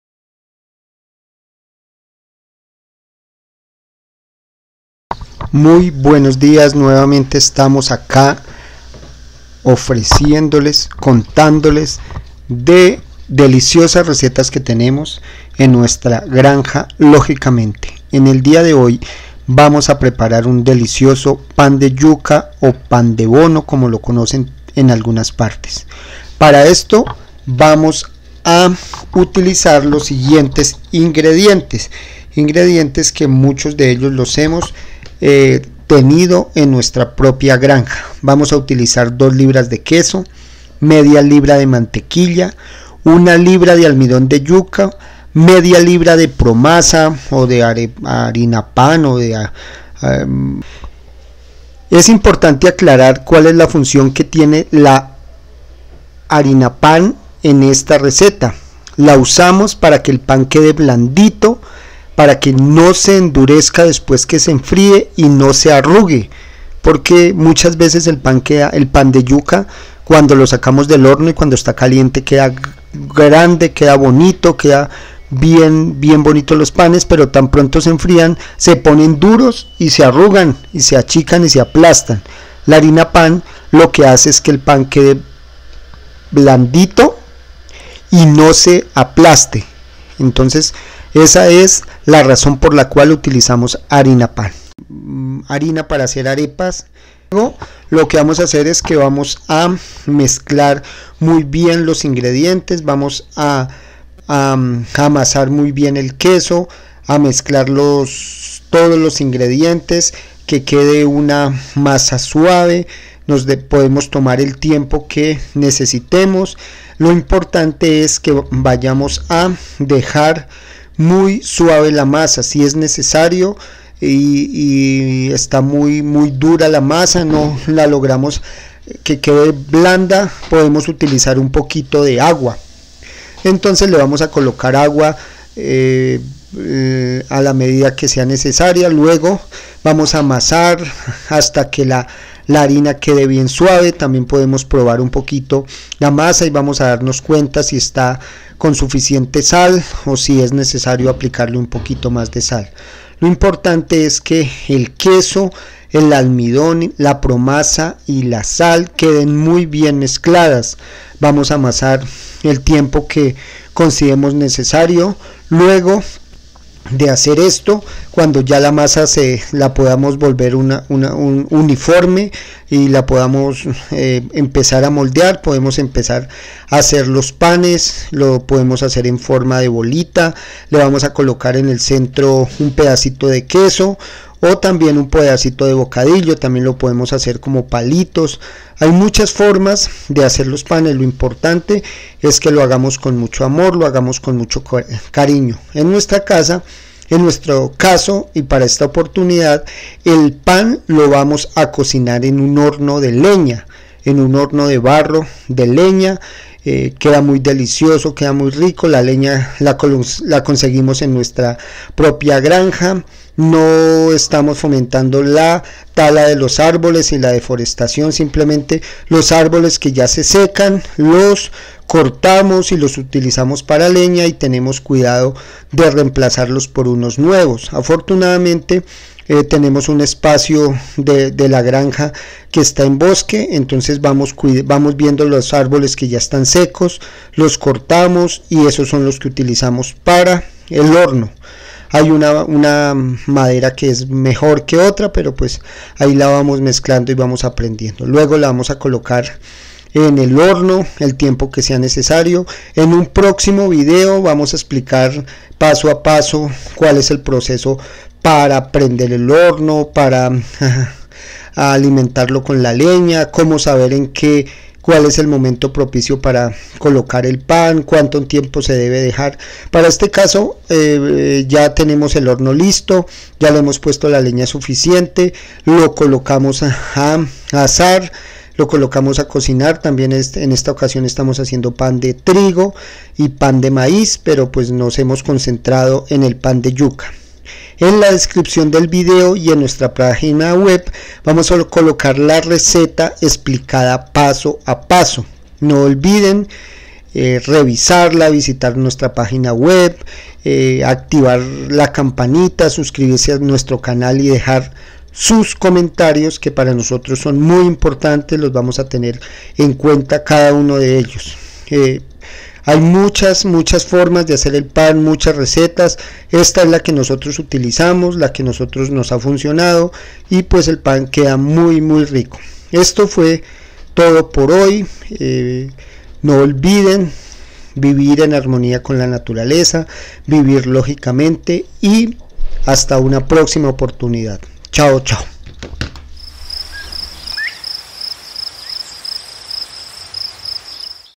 Paso a paso para hacer un delicioso pan de yuca
Como hacer un delicioso pan de yuca. Para el día de hoy  les mostraremos el paso a paso para hacer un rico pan en nuestra casa, no olviden visitar nuestra pagina web donde encontraran el paso a paso para esta deliciosa receta, dejar sus comentarios y seguirnos en nuestras redes sociales.

INGREDIENTES
2 libras de queso
½ de mantequilla.
1 libra de almidón
½ libra de promasa,
sal al gusto
PREPARACION

En un recipiente colocar el queso, la mantequilla   con la sal y mezclarlo muy   bien para que no queden grumos, luego se agrega  un poco de agua, seguir amasando hasta que quede uniforme  y suave, luego se colocan lentamente, los otros ingredientes (almidón y   promasa) se sigue amasando lo suficiente hasta que la masa quede uniforme, una buena prueba es hacer un palito de masa y tratar de unirlo para que quede un circulo si no se parte ya esta lista la masa, luego de esto se forman los panes en la forma que lo prefiramos y se ya se puede hornear.